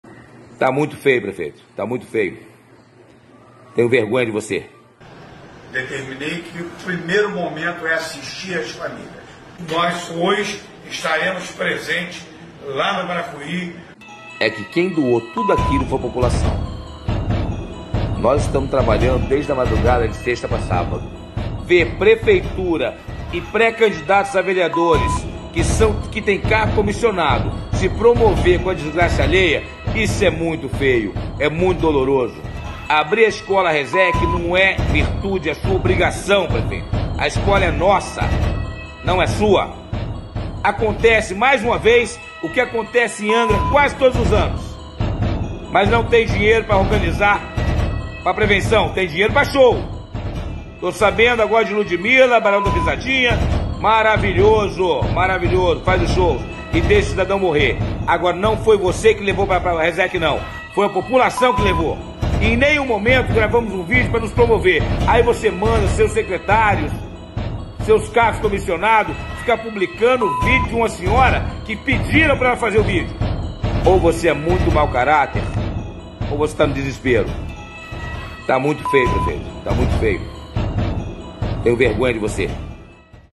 🤬🗣 "Ou você é MAU CARÁTER ou tá no DESESPERO! Diz morador ao prefeito de Angra.
Morador relata fatos e manda recado ao prefeito de Angra, Fernando Jordão. A realidade na tragédia do bairro Bracuy em Angra.